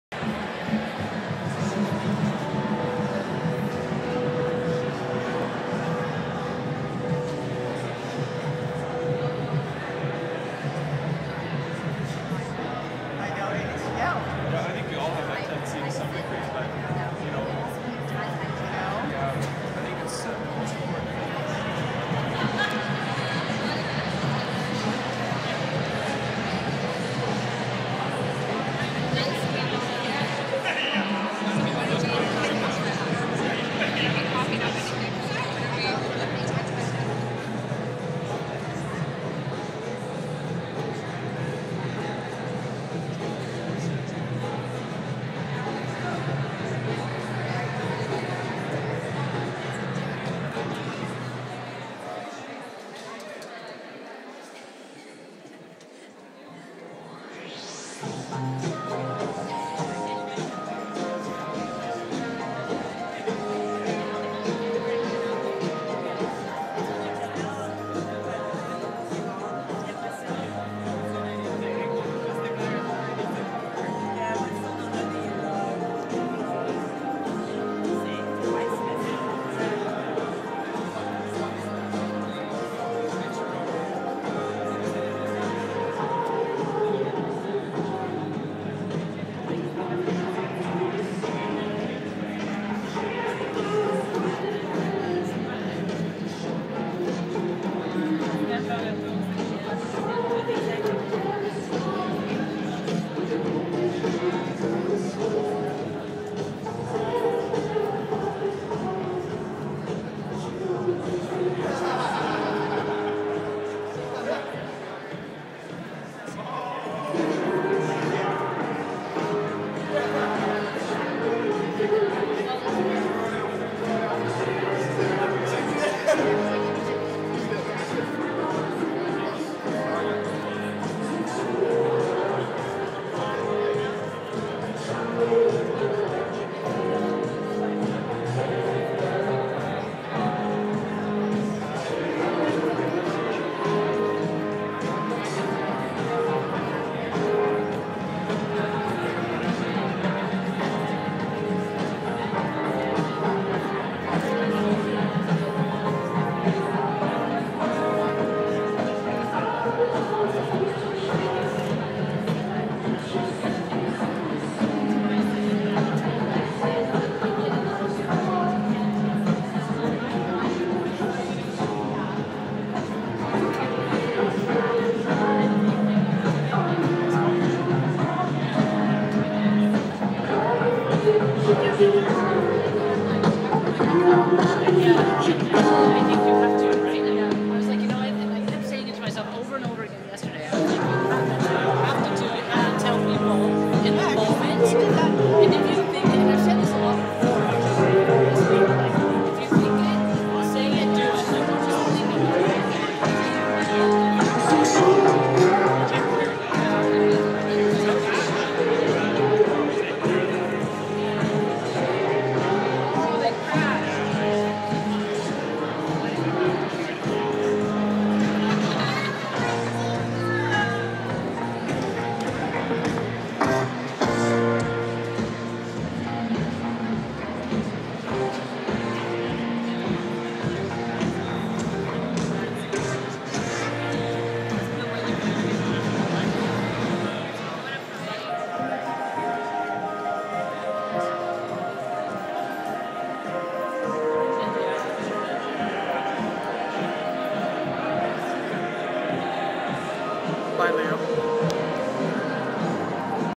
Goodbye Leo Obstbaum
VANOC hosted a touching celebration of Leo Obstbaum's life and showcased the work he and his team had produced so far for the Olympics. Impressive.